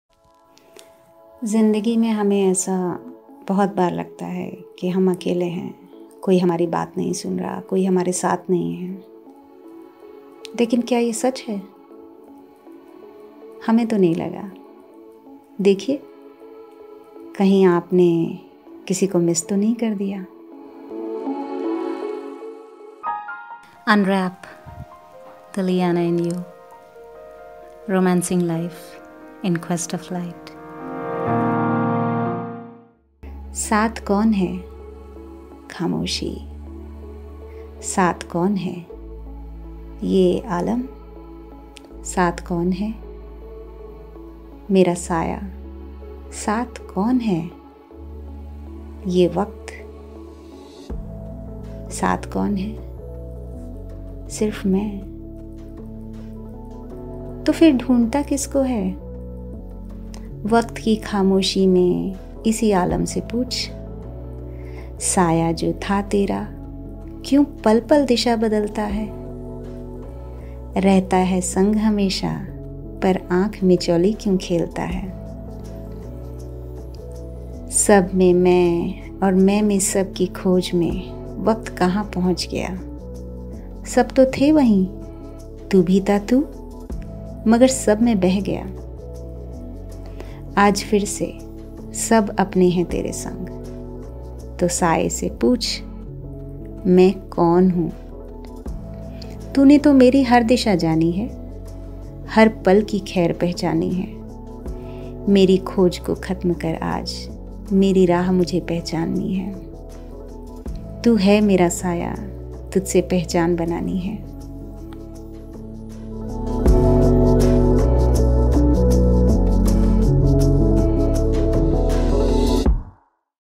Being Alone | Khud ko akela na samjhe
There are times in our life when we feel that we are alone, no one is there with us to support us, sit with us and talk to us. But we are wrong. We are never alone. In this poetry, we have emphasized the fact that we are not alone. Let's find out who is with us even when we feel that we are alone.